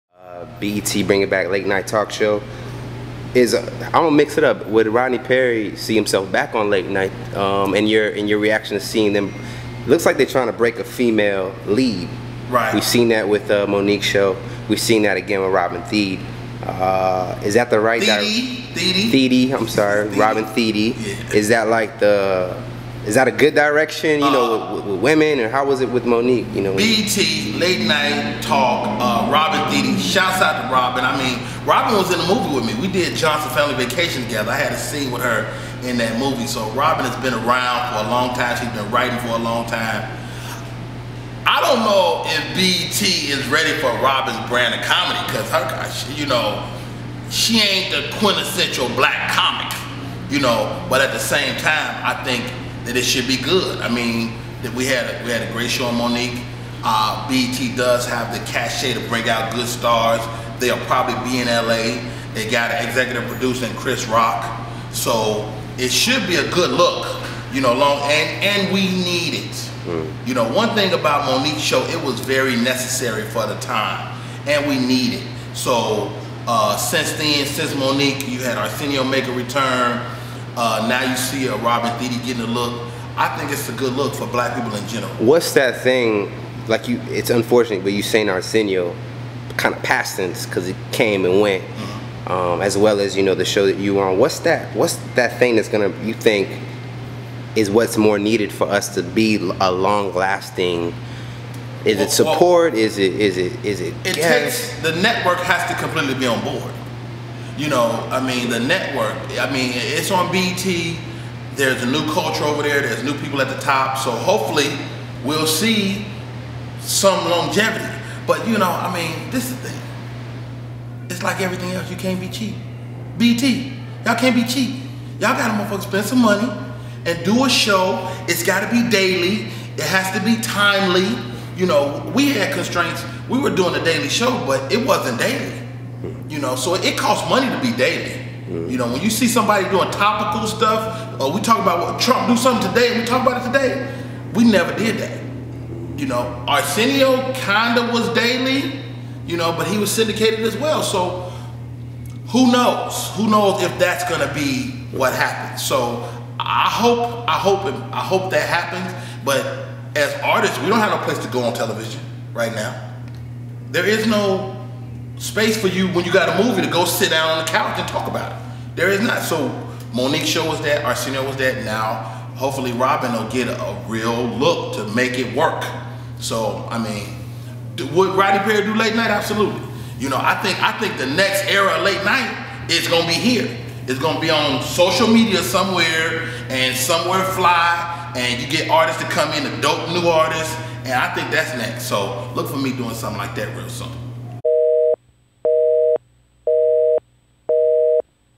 A Black Successful Late Night TV Show Will Need Money - Rodney Perry
- In another part our talk with Rodney Perry, he discuss black late night TV shows, his time on The Monique Show, and more.